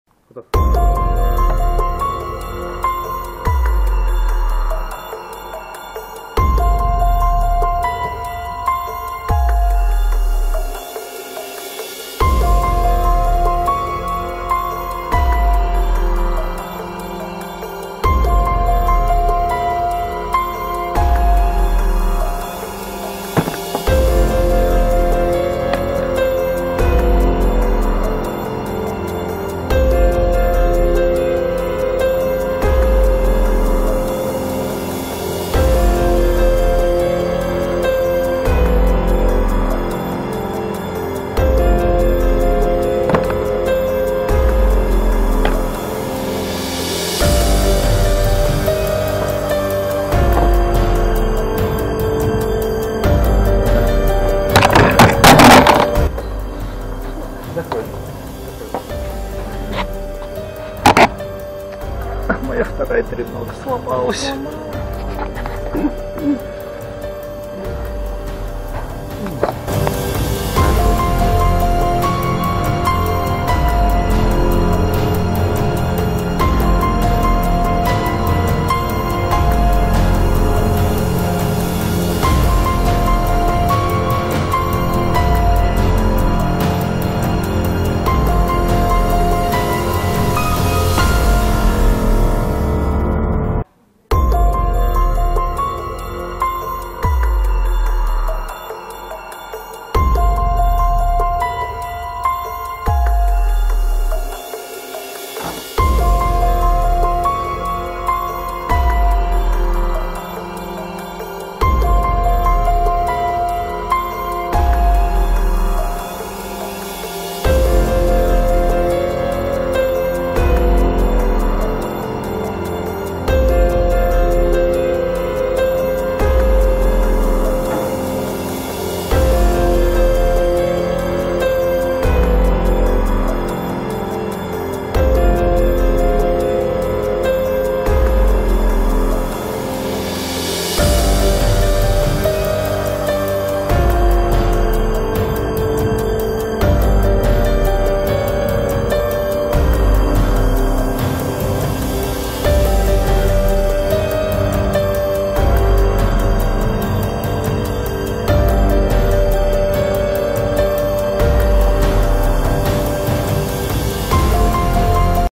2x2x3 average of 5 - 7.05 (official solves)
Достаточно удачные сборки и очень неудачная съемка. После пары попыток судья Варвара случайно смахнула камеру со стола, тренога при этом сломалась (претензий к судье нет, это очень милая девочка, хорошо помогала весь чемпионат). Я попросил волонтера Олега поснимать оставшиеся сборки с рук, но от волнения в одной сборке он слишком рано остановил запись, а в остальных не смог унять дрожь в руках, но в любом случае выручил.
В результате я занял третье место в этой дисциплине и улучшил свое среднее время. Возможно, проблемы с камерой добавили мне адреналина, все закончилось успешно.

Tambov Autumn 2019